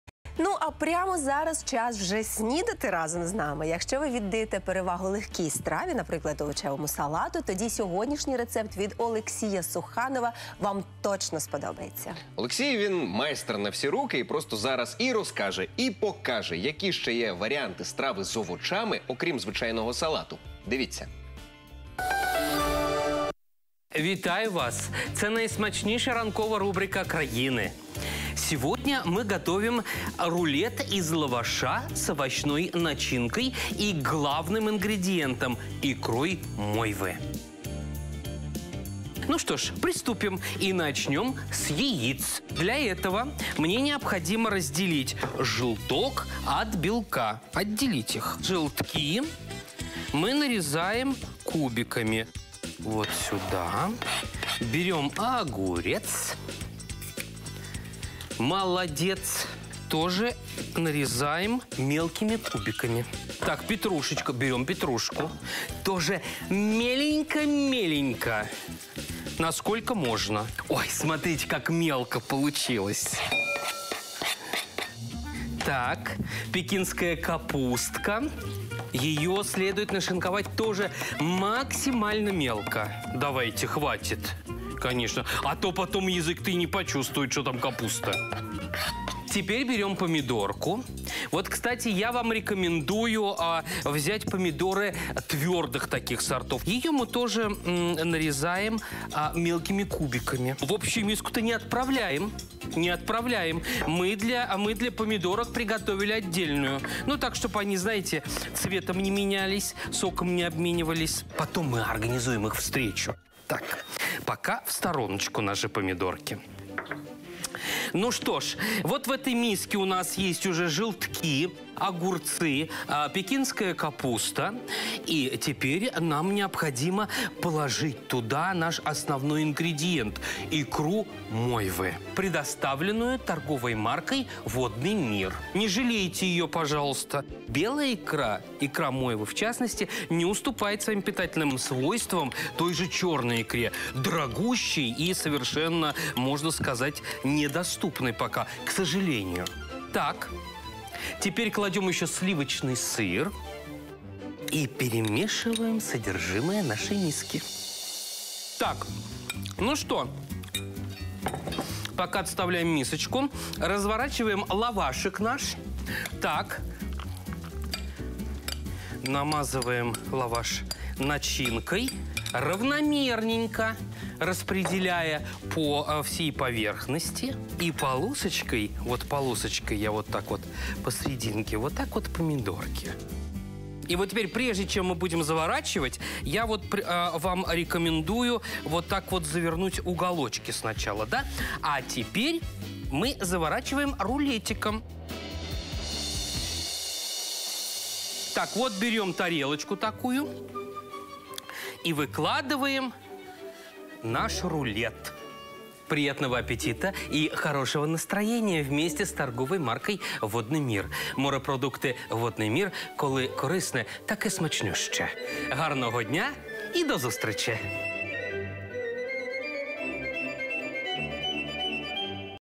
Рулет з лаваша з ікрою мойви
Готуємо рулет з лаваша з ікрою мойви ТМ "Водный мир",  разом з Олексієм Сухановим